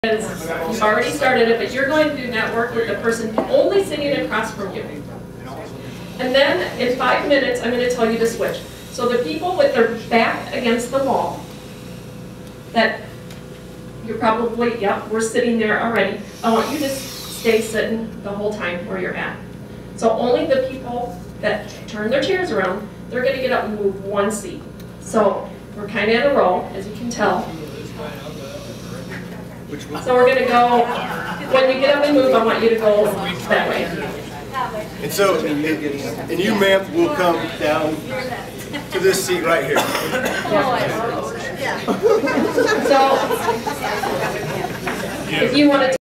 Networking Event - IaREIA & Iowa Landlord Assn - Winter Conference 2014
Sherri Kubly, founder of the NE Iowa Landlord Assn group meetings in Manchester, Iowa (which meet on the 3rd Tuesday of the month), put together a great networking event. Here she is introducing the event and how rotations will work during the session.   She was awesome in this role, as were our members and attendees!